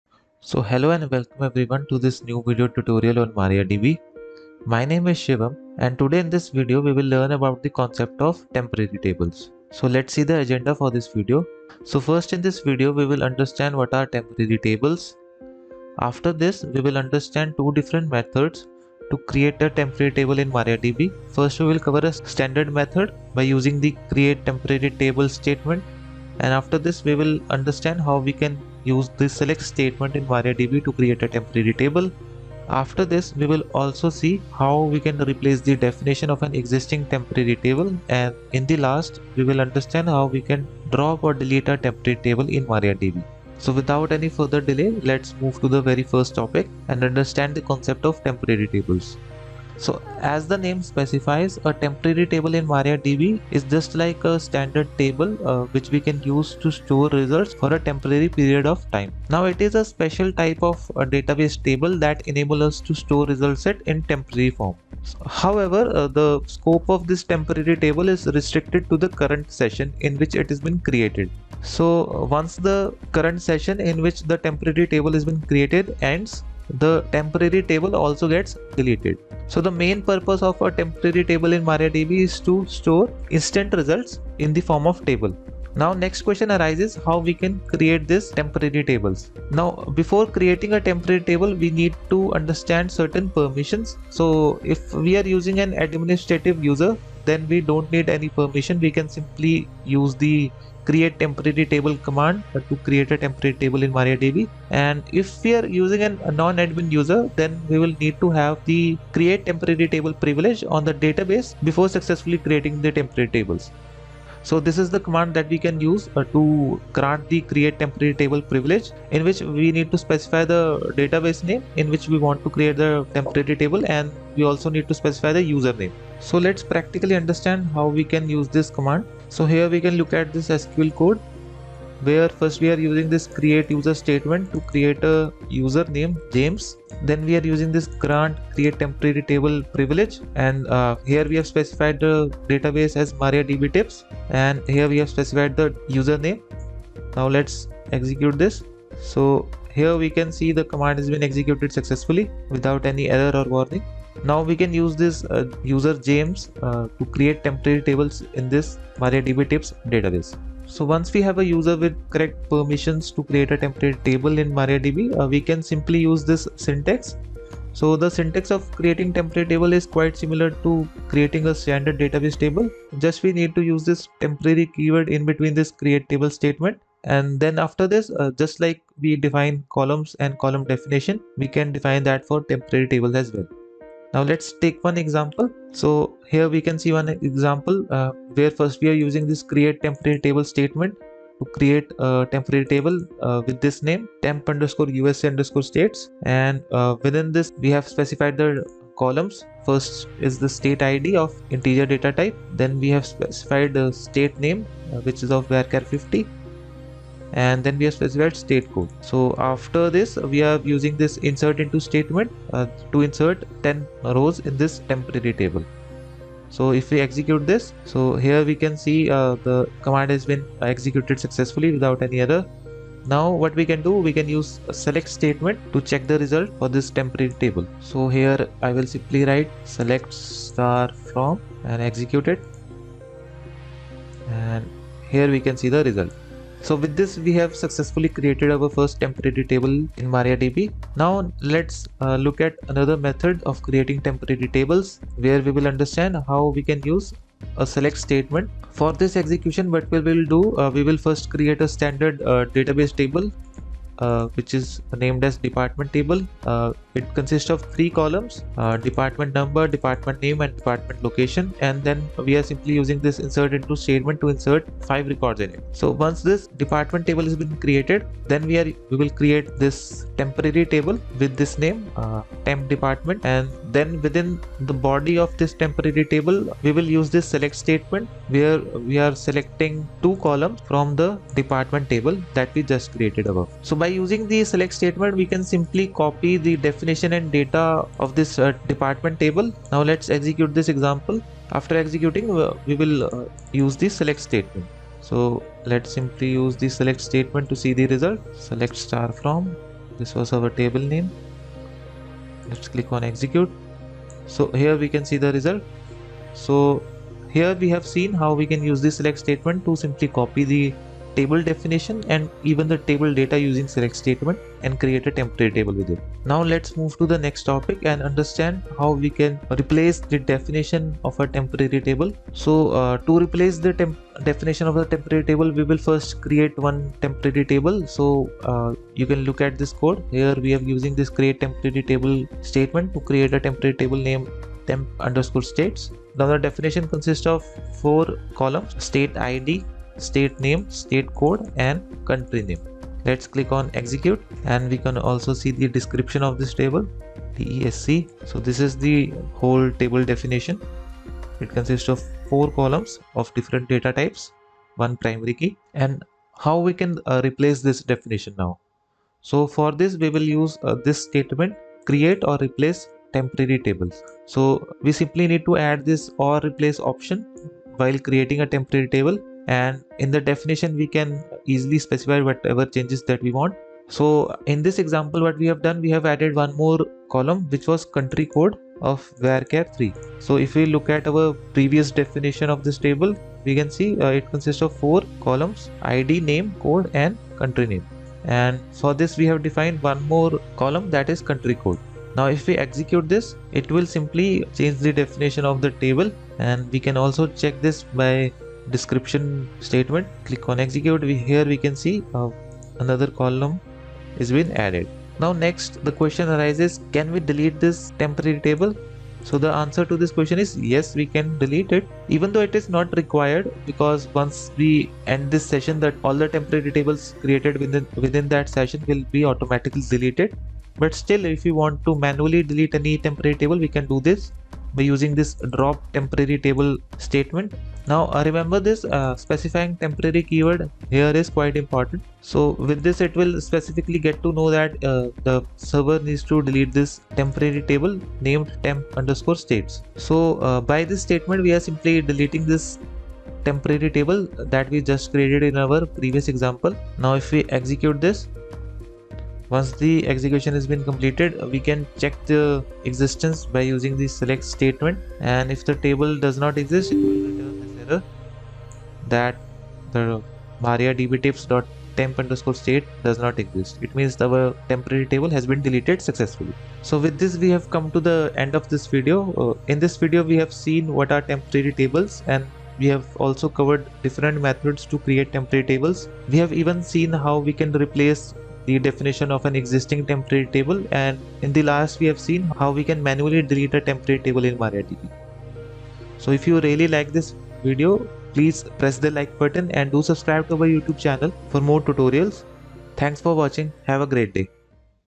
Temporary Table in MariaDB
In this MariaDB video tutorial, I have explained everything about Temporary Tables in MariaDB or MariaDB Temporary Table. I have explained here what is a temporary table in MariaDB, how to create a temporary table in MariaDB, etc. how to delete a temporary table in MariaDB.
Here is the list of queries that I will cover
1. What is a temporary table in MariaDB
2. Create a temporary table in MariaDB
3. Create a temporary table in MariaDB using a SELECT statement
4. Replace the definition of the temporary table in MariaDB
5. Delete/Drop a temporary table in MariaDB